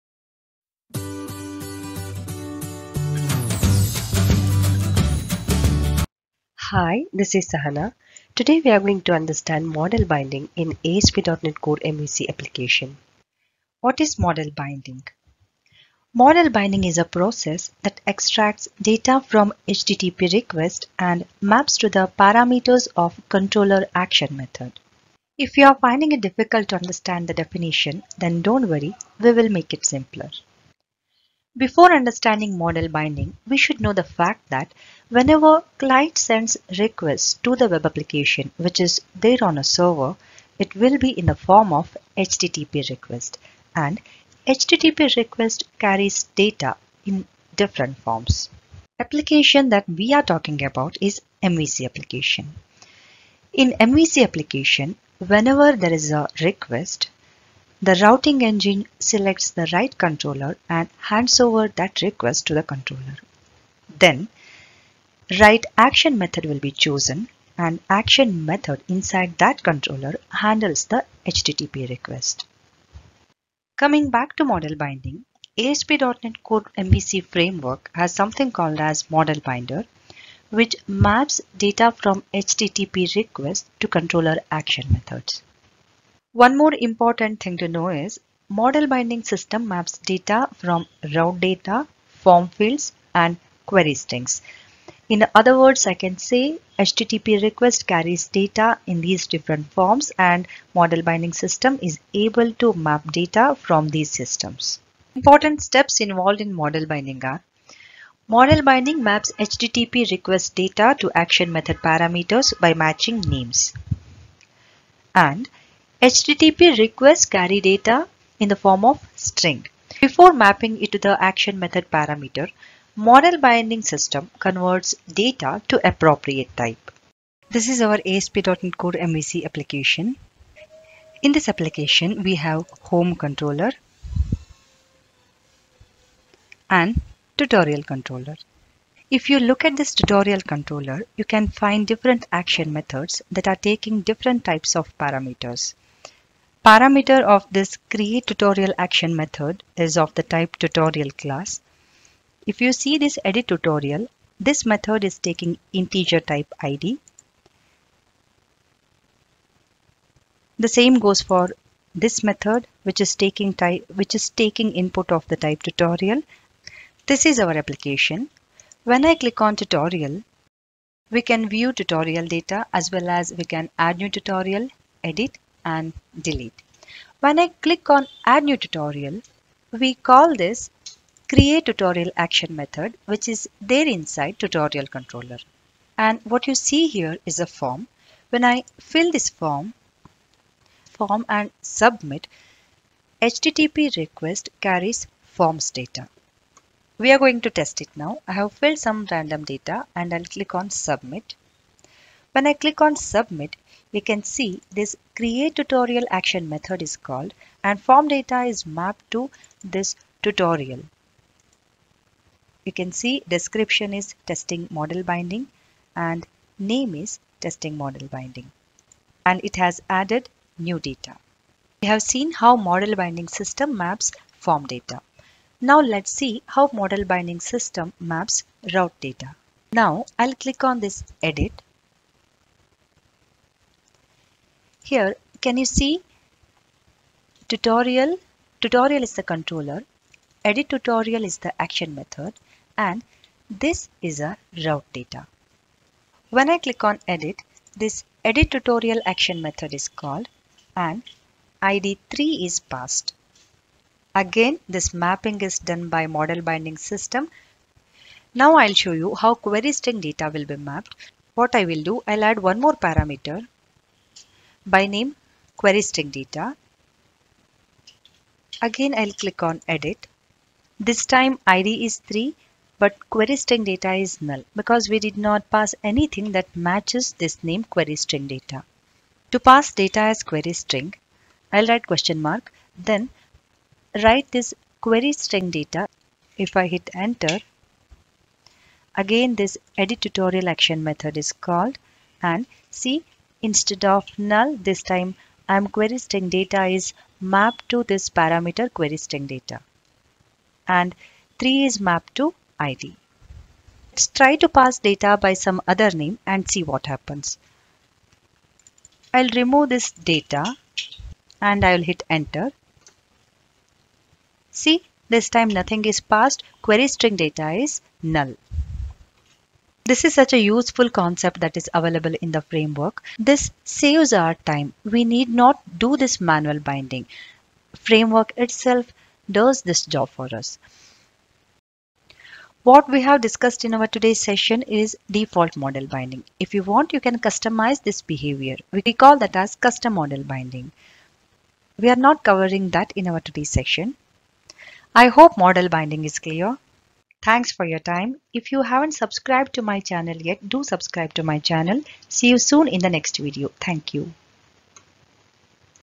Model Binding in asp.net core MVC | Asp.net core MVC 6.0 tutorial for beginners
Previous video:
How to implement CRUD operations in asp.net core MVC

In this video you will understand :
# What is model binding in asp.net core MVC application?
# How model binding works in asp.net core MVC?
# Steps involved in model binding